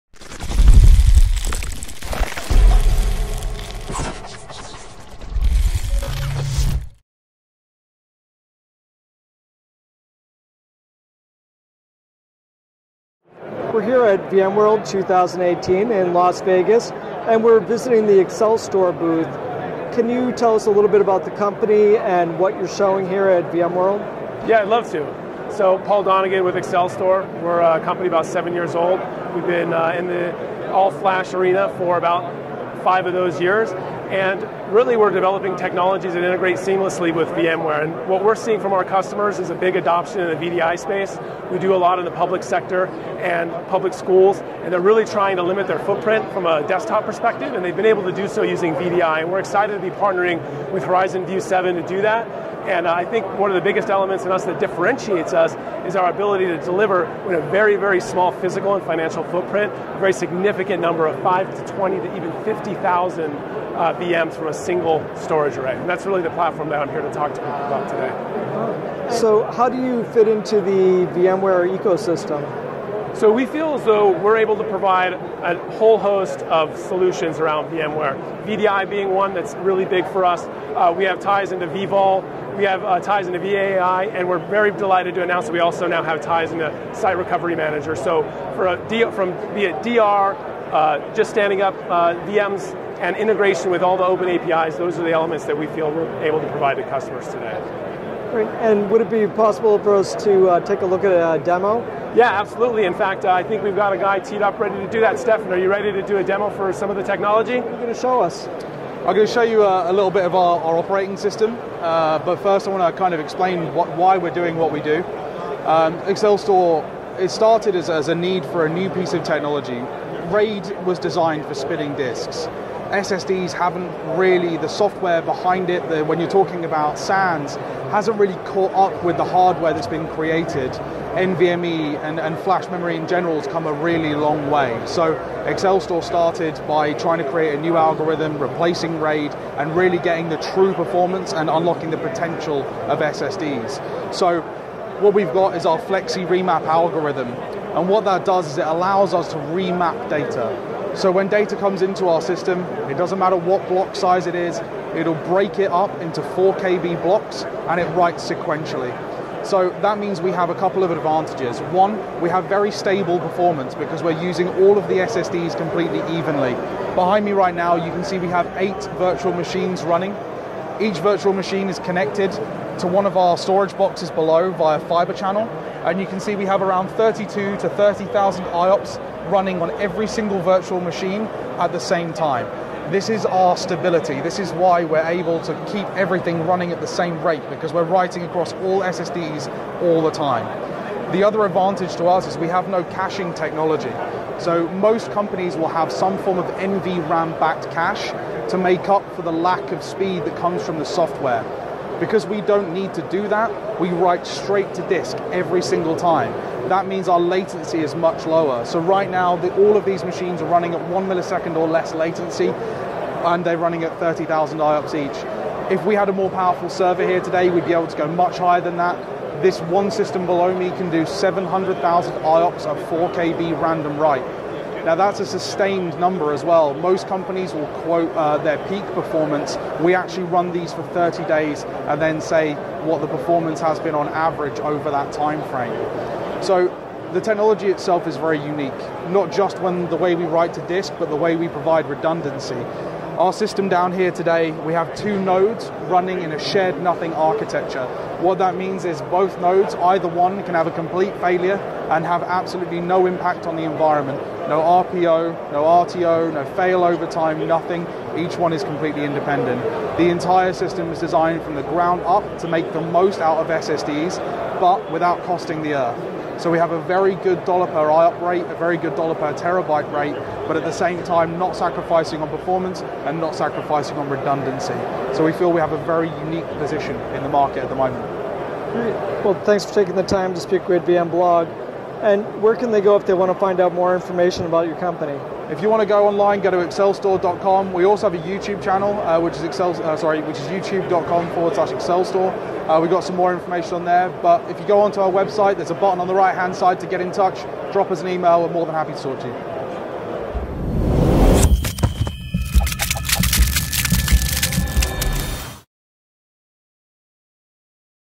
Accelstor VMworld 2018 - VMblog Interview
VMblog visits the Accelstor booth during VMworld 2018 in Las Vegas.

The company is accelerating the paradigm shift from conventional disk arrays to modern all-flash storage.